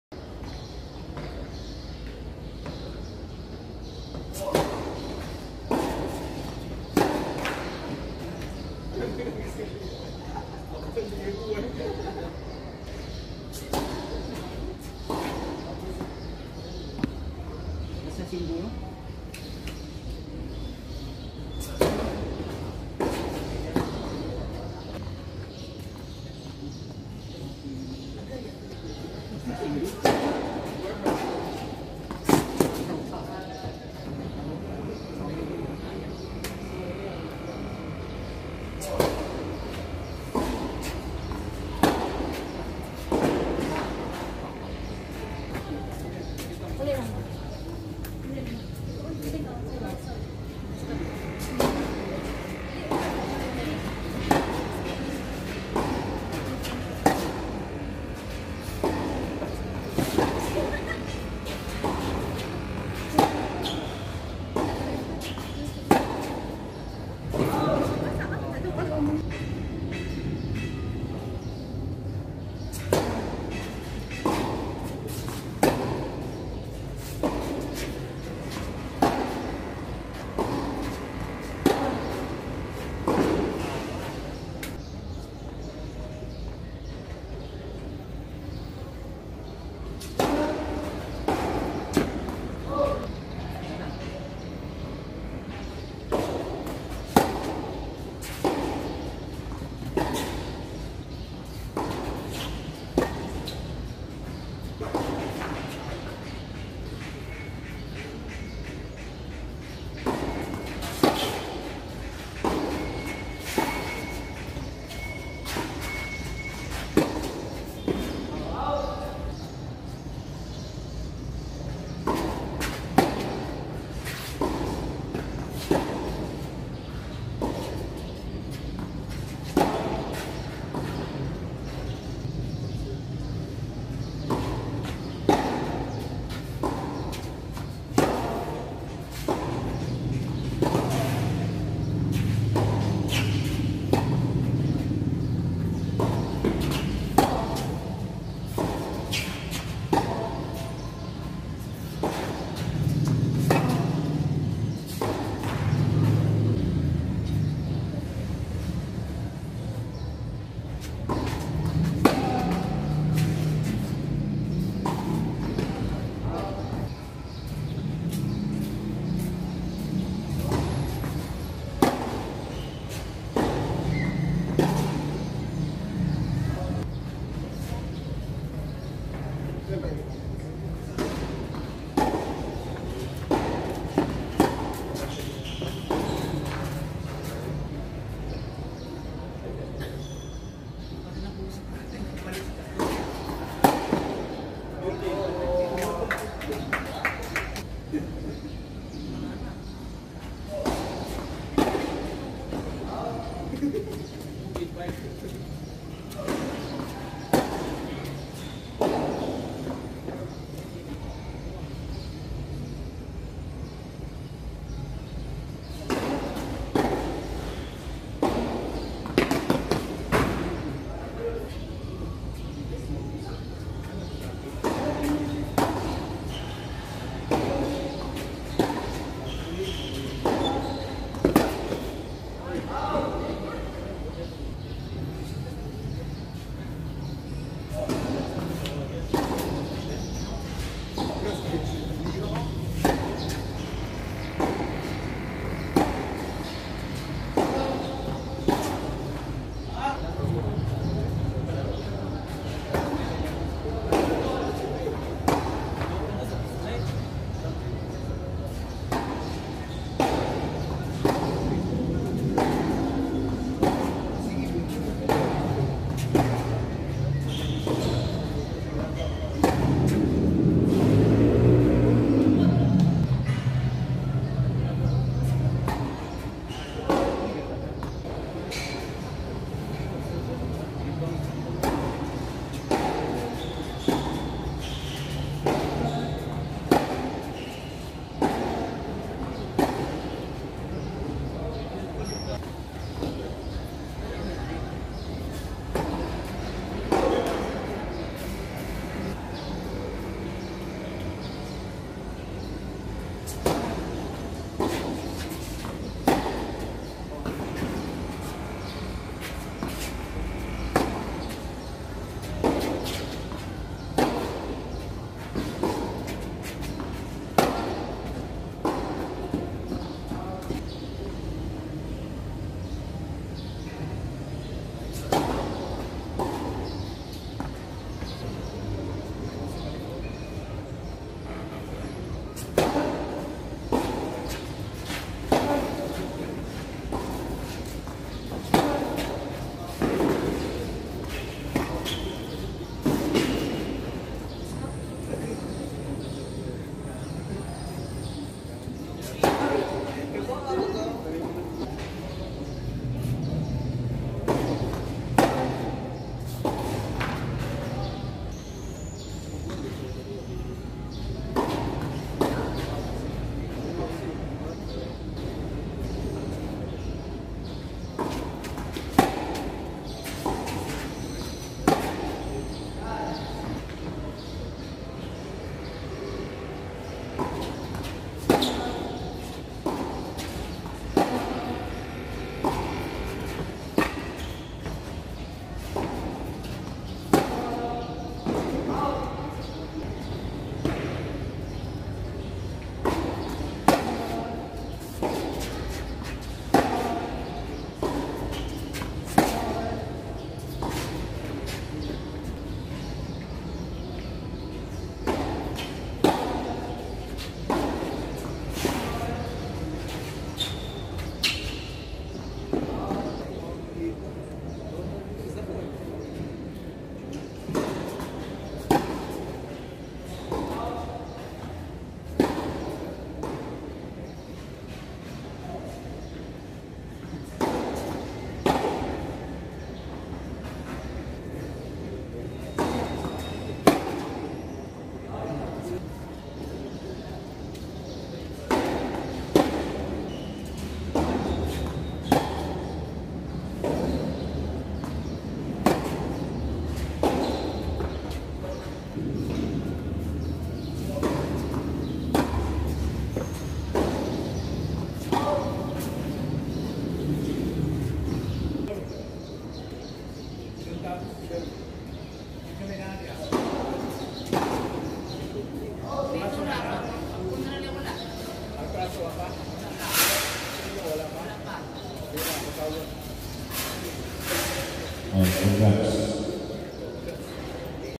Christopher Encarnacion vs Josshua Kinaadman
City of Naga tournament Semi-Finals.

Located at Naga City, Cebu.